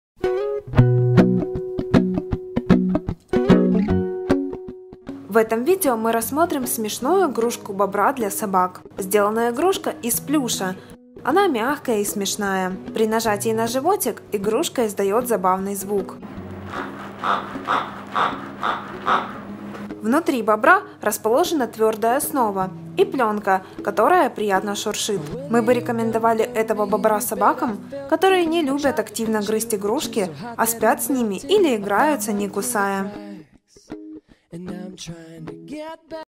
Trixie ТХ 35910 плюшевая игрушка Бобер для собак 27см
Ваша собака с удовольствием поиграет милым бобром Trixiе. Игрушка изготовлена из приятного на ощупь плюша. Благодаря удлиненной форме игрушки собаке удобно носить ее в зубах.